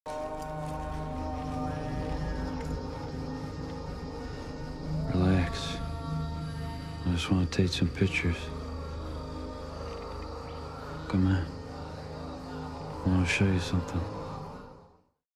"I just wanna take some bull moose pictures..."
A bull moose in northern Teton Park 10/5/22